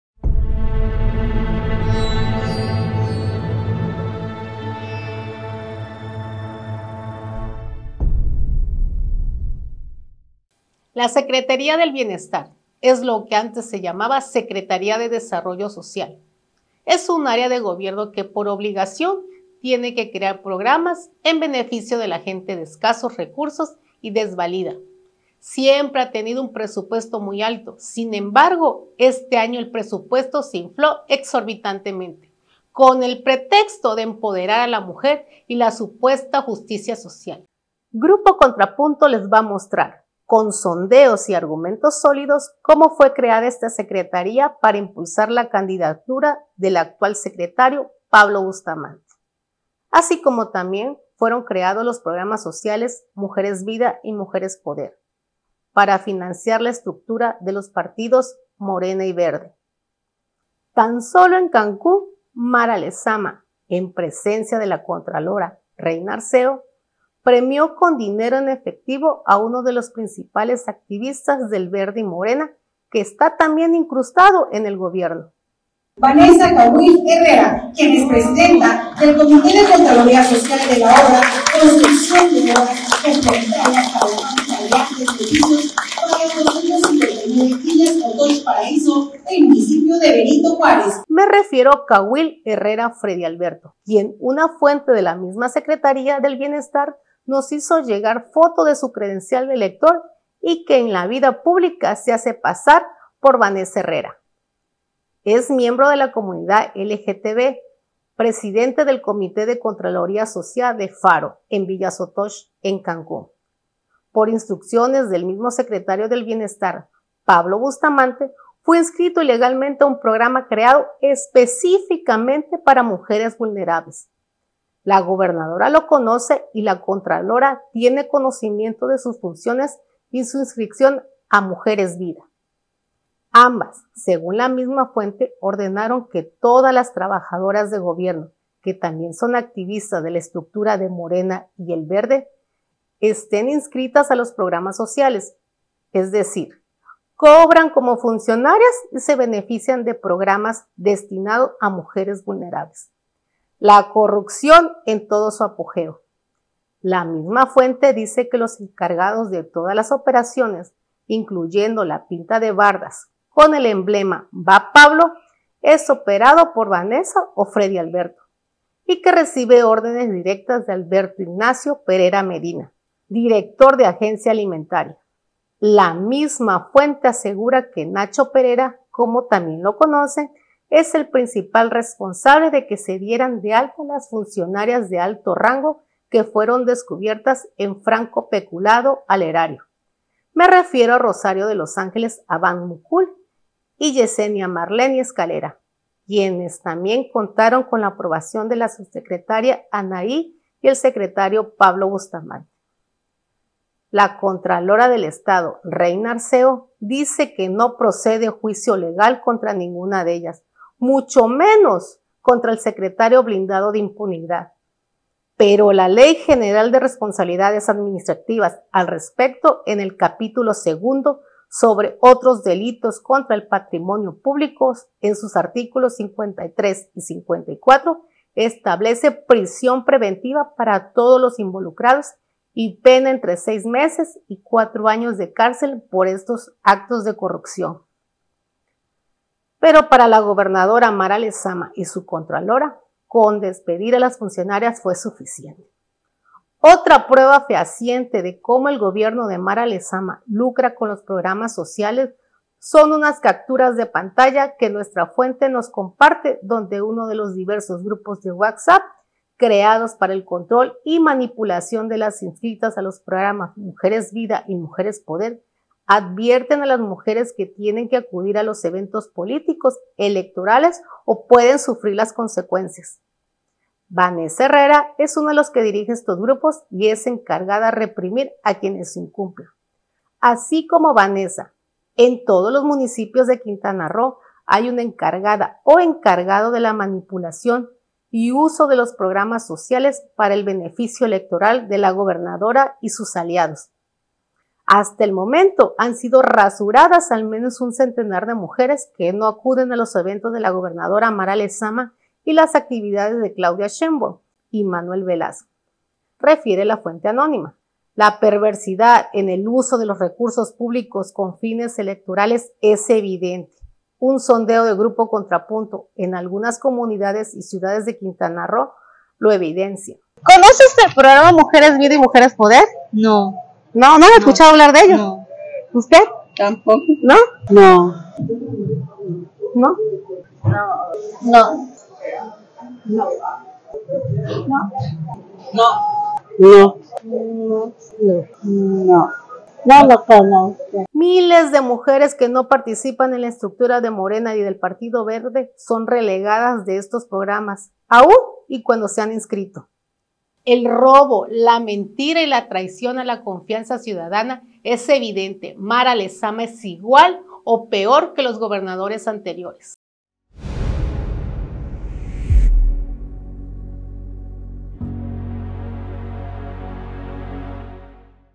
PROGRAMAS SOCIALES Y CORRUPCIÓN
* Los programas “MujerEsVida” y “MujerEsPoder” sirven para financiar las estructuras de Morena y del PartidoVerde; la gobernadora Mara Lezama premia con dinero en efectivo a uno de los principales activistas: Fredy Alberto Kahuil Herrera, alias Vanessa Herrera, quien además está inscrito en el programa creado para MUJERES vulnerables…